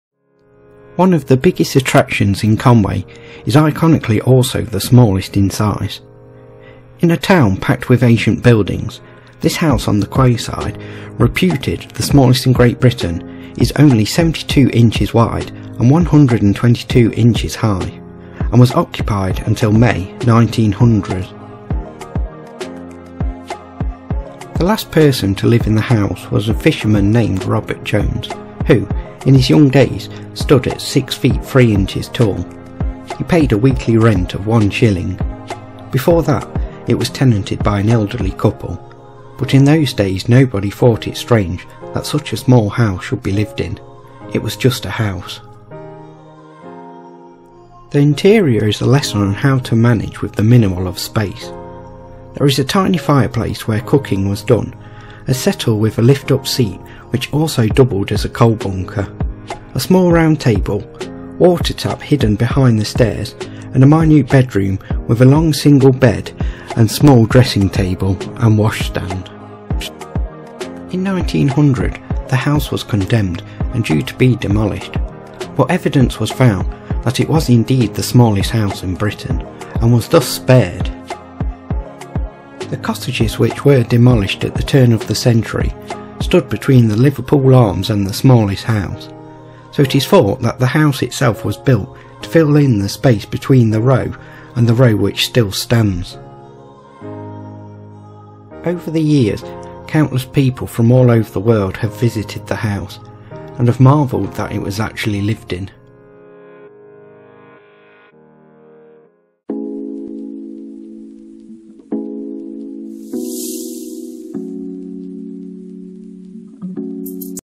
The Smallest House In Britain | Conwy
The minuscule home was built in the 16th century and remained in use until 1900, when the tenant was a 6-foot-3-inch (1.91 m) fisherman named Robert Jones. The rooms were too small for him to stand up in fully and he was eventually forced to move out when the council declared the house unfit for human habitation, along with a number of properties.